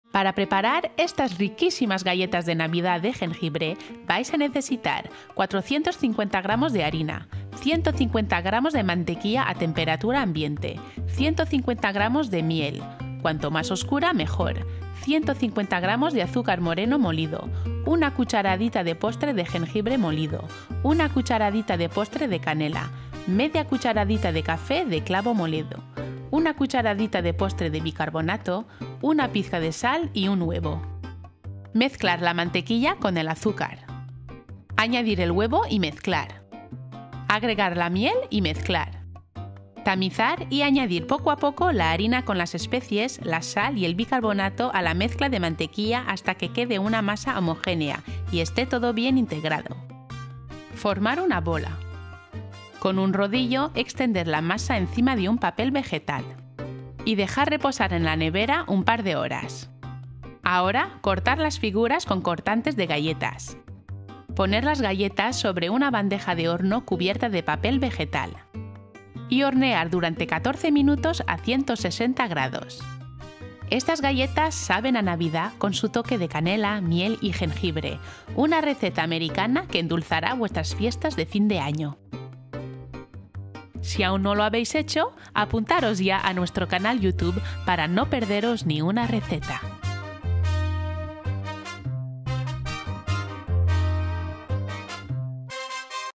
Receta de las galletas de navidad de jengibre
Estas navidades preparad galletas de navidad de gengibre ! Están tan buenas...y ese toque a gengibre...de escándalo! La masa lleva mantequilla y especies como gengible y canela y también lleva miel y azúcar moreno. Esta mezcla hace que estas galletas de navidad de gengibre sean tan buenos y con un sabor muy especial que sabe a navidad! Una receta americana fácil y rápida de hacer. Y si tenéis niños no dudéis en preparar estas galletas de navidad con ellos! Se lo pasarán muy bien amasando la masa y dándoles forma con cortantes de galletas. Venga, en cocina y a por estas maravillosas galletas de navidad de genbibre americanas !

00:00 : Ingredientes
00:32 : Receta de las galletas de navidad de jengibre
01:14 : Conclusión

Ingredientes para las galletas de navidad de jengibre:

- 450 gr de harina
- 150 gr de mantequilla a temperatura ambiente
- 150 gr de miel cuanto más oscura mejor
- 150 gr de azúcar moreno molido
- 1 cucharadita de postre de jengibre molido
- 1 cucharadita de postre de canela
- ½ cucharadita de café de clavo molido
- 1 cucharadita de postre de bicarbonato
- 1 pizca de sal
- 1 huevo